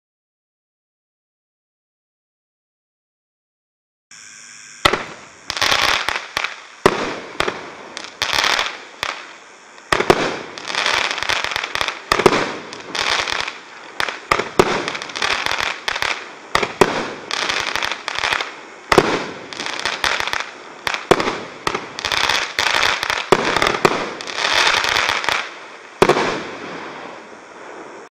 M609 DARE DEVIL
Great hexagon 200g cake! Blue with chrysanthemum mine up to big gold plam with red strobe/blue/white strobe. 3 shots finale! Visual Delight!